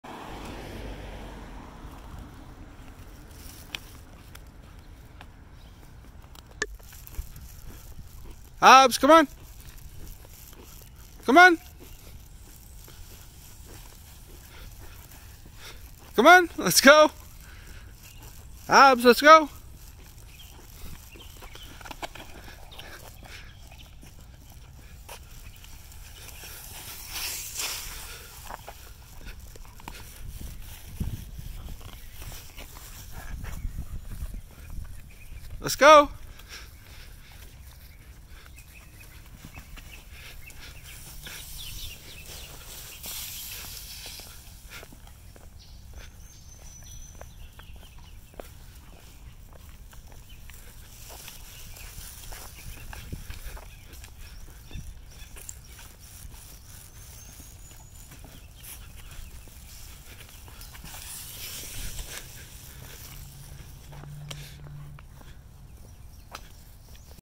Hobbes the Wheaten Terrier puppy 10 months old playing with a palm frond.
Hobbes is a 10 month old Wheaten Terrier puppy who loves to play while we are out for a walk.  He loves to drag huge palm fronds around.